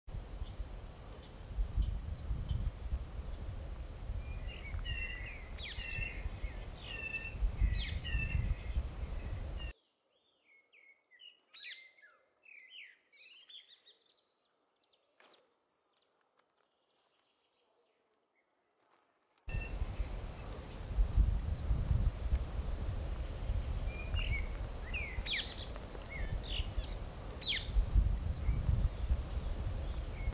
Un sedentario como el Pinzón Vulgar y un migrante como la Golondrina
Durante el invierno la Península recibe abundantes contingentes de Pinzones Vulgares procedentes de Europa, sin embargo nuestras Golondrinas Comunes migran más al sur, aunque algunas como las que he encontrado, siguen alimentándose de insectos en una laguna